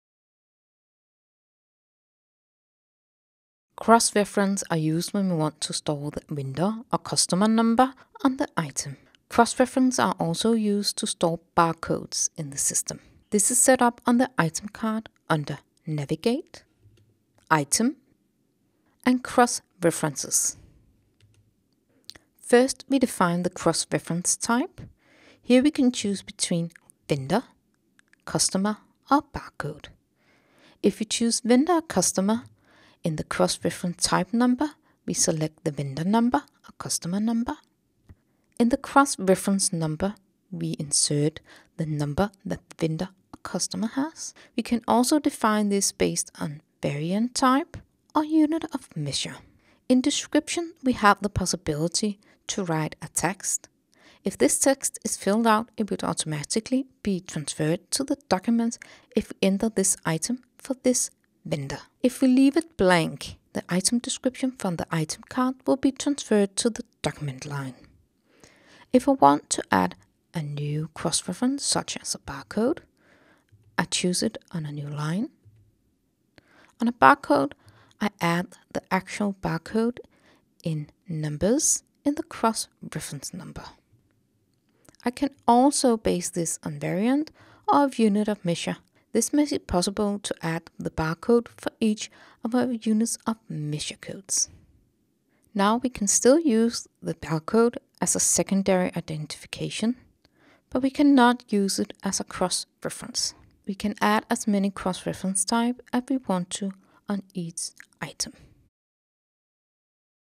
Item References and barcodes - Microsoft Dynamics 365 Business Central
How to use cross references and bar codes in Business Central? Cross-reference are used, when we want to store the vendor or customer number on the item. [ENG19042404]